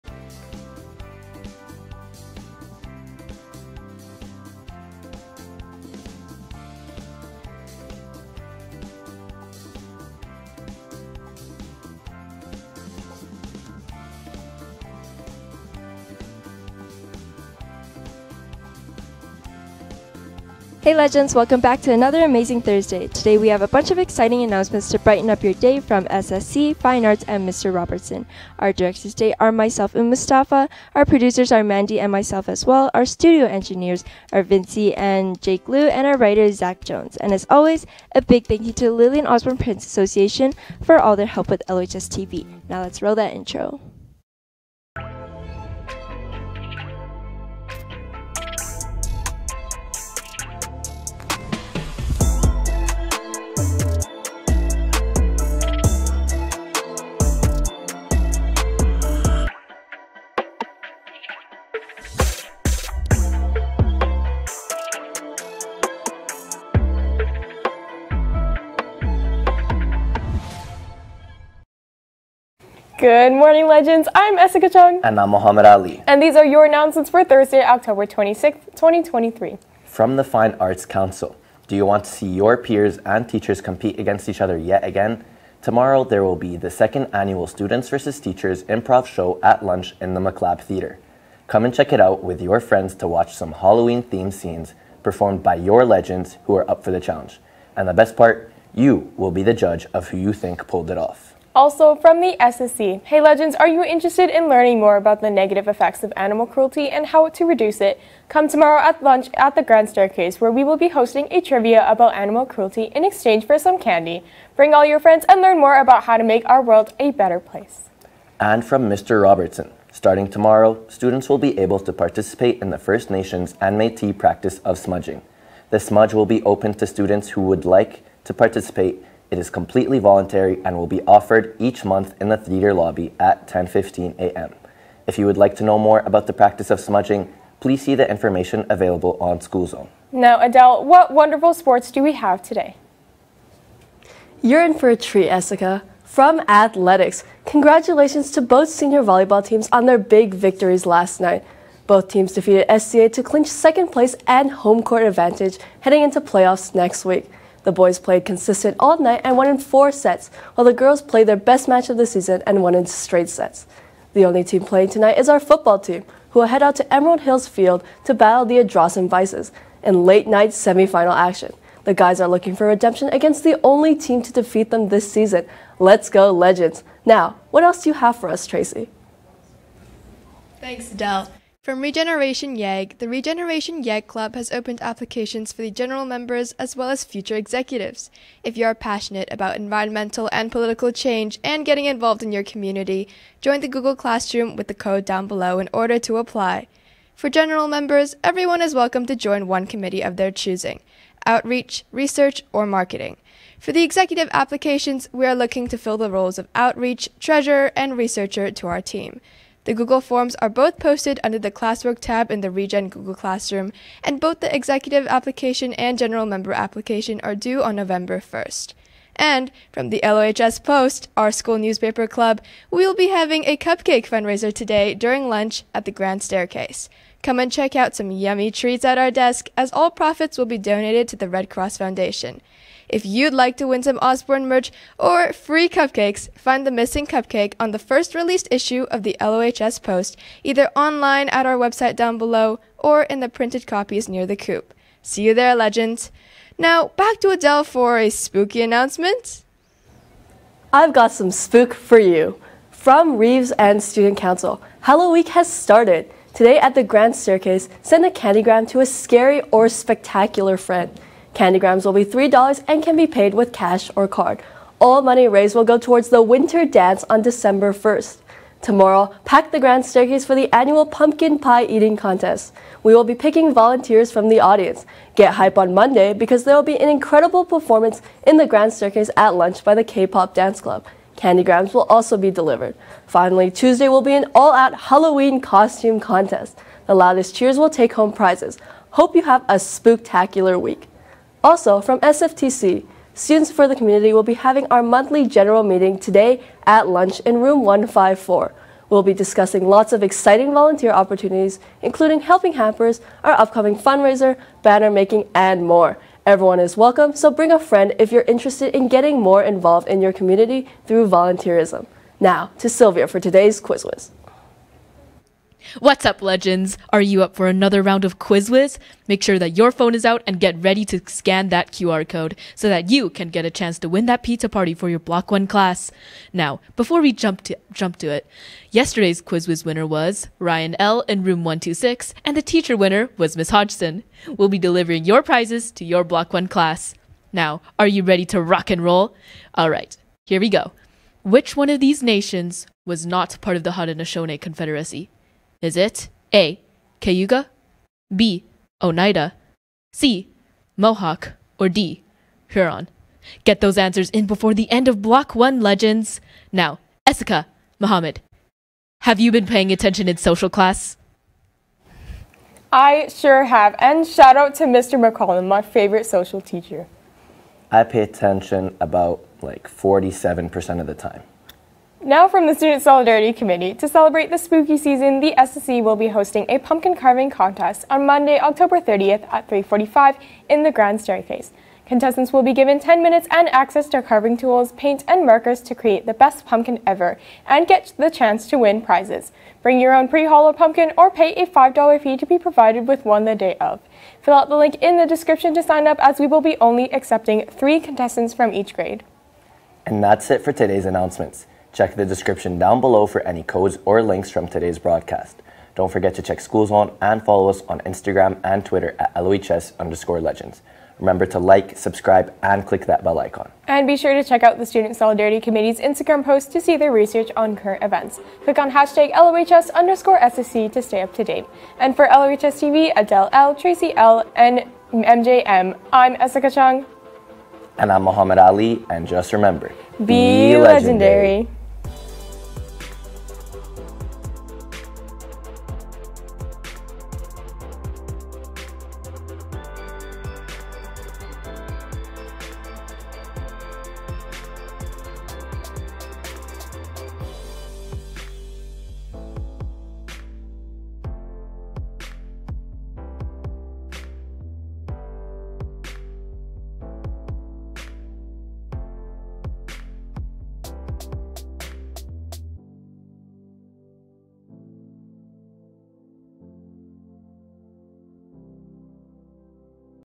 ANNOUNCEMENTS OCTOBER 26th, 2023
Regenyeg: [ycjukzj]
LohsPost: [lohs.ca/lohspost]
These are your announcements for Thursday, October 26th 2023
From the Fine Arts Council: Tomorrow there will be the second annual students vs teachers improv show at lunch in the Maclab Theatre! YOU will be the judge of who you think pulled off the best halloween themed scene.
Also, From the SSC: Hey legends, are you interested in learning more about the negative effects of animal cruelty and how to reduce it? Come tomorrow at lunch at the grand staircase where we will be hosting a trivia about animal cruelty in exchange for some candy! Bring all your friends and learn more about how to make our world a better place!
From MR. ROBERTSON: Starting tomorrow, students will be able to participate in the First Nations and Metis practice of smudging. The smudge will be open to students who would like to participate, is completely voluntary, and will be offered each month in the Theatre Lobby at 10:15 am. If you would like to know more about the practice of smudging, please see the information available on SchoolZone.

FROM ATHLETICS - Congratulations to both Sr Volleyball teams on their big victories last night. Both teams defeated SCA to clinch 2nd place and home court advantage heading into playoffs next week. The boys played consistent all night and won in 4 sets while the girls played their best match of the season and won in straight sets.
The only team playing tonight is our football team who will head out to Emerald Hills Field to battle the Ardrossan Bisons in late night semi-final action. The guys are looking for redemption against the only team to defeat them this season. Let’s go Legends!!

FROM REGENERATION YEG: The Regeneration YEG club has opened applications for the general members as well as future executives! If you are passionate about environmental and political change and getting involved in your community, join the google classroom with the code down below in order to apply [ycjukzj]! For general members: Everyone is welcome to join one committee of their choosing: Outreach, Research, or Marketing. For the Executive Applications: We are looking to fill the roles of Outreach, Treasurer, and Researcher to our team. The google forms are both posted under the "Classwork" tab in the ReGen google classroom. Both the Executive Application and General Member Application are due on November 1st.

And FROM THE LOHS POST, OUR SCHOOL NEWSPAPER CLUB: We will be having a cupcake fundraiser today during lunch at the grand staircase.All profits will be donated to the Red Cross Foundation! If you’d like to win some Osborne merch or free cupcakes, find the missing cupcake on the 1st released Issue of the LOHS Post either online at our website down below (lohs.ca/lohspost) or in the printed copies near the COOP!

FROM REEVES & STUDENT COUNCIL: Today at the grand staircase, send a candy gram to a scary or spectacular friend! Candy grams will be $3 and can be paid with cash or card. All money raised will go towards the WINTER DANCE on December 1st! TOMORROW pack the grand staircase for the annual pumpkin pie eating contest! We will be picking volunteers from the audience! Get hype on MONDAY because there will be an incredible performance in the grand staircase at lunch by the K-POP dance club! Candy grams will also be delivered. Finally, TUESDAY will be an all out Halloween costume contest! The loudest cheers will take home prizes! Hope you have a spooktacular week!

Also From SFTC: Students for the Community will be having our monthly General Meeting today, at lunch in Room 154! We will be discussing lots of exciting volunteer opportunities including Helping Hampers, our upcoming fundraiser, banner making and more! Everyone is welcome!

SILVIA: QUIZWIZ TIME! Scan that QR code, so that you can get a chance to win that pizza party for your block 1 class! Now before we jump at it, yesterday’s QuizWiz winner was: Ryen.L in Room 126. And the Teacher winner was Ms Hodgson. We’ll be delivering your prizes to your Block 1 class.

Now are you ready to rock and roll? Alright here we go,
Which of these nations was not part of the Haudenosaunee confederacy?
Is it…A. Cayuga, B. Oneida, C. Mohawk, or D. Huron

Get those answers in before the end of Block 1 Legends.

ESSICA: Now FROM THE STUDENT SOLIDARITY COMMITTEE: to celebrate the spooky season, the SSC will be hosting a pumpkin carving contest on Monday, October 30th, at 3:45pm in the grand staircase! Contestants will be given 10 minutes and access to carving tools, paint, and markers to create the best pumpkin ever, and get a chance to win prizes! Bring your own pre-hollowed pumpkin, or pay a $5 fee to be provided with one the day of.